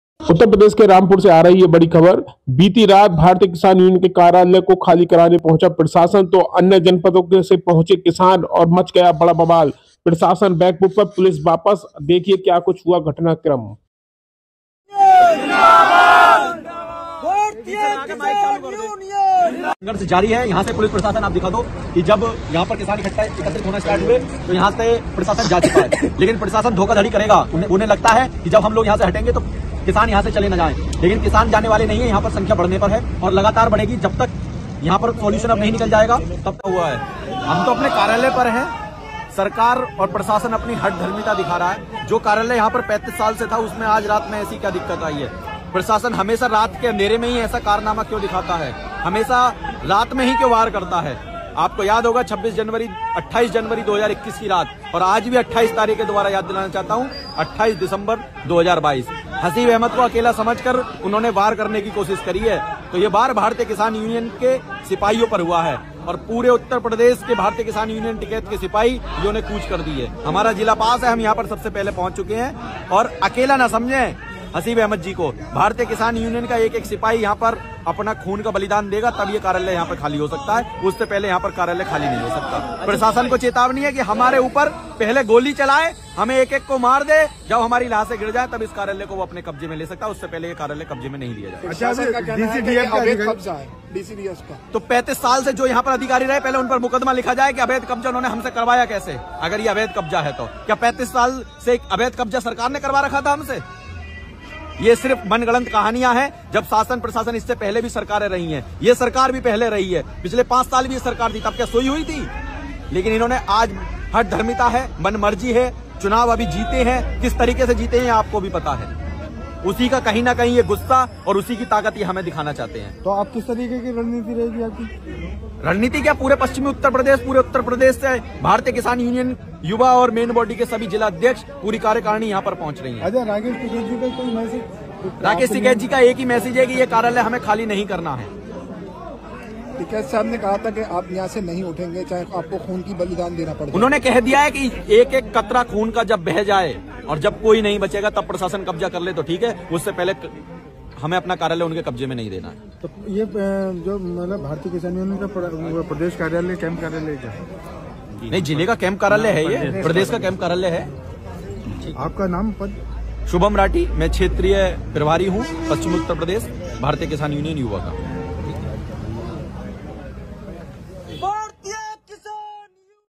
Rampur में BKU और प्रशासन में बड़ी झड़प कार्यालय खाली कराने को लेकर हंगामा
प्रशासन और भाकियू कार्यकर्ताओ में बड़ा बबाल कार्यालय खाली कराने को लेकर हंगामा
bku in rampur
rampur me bku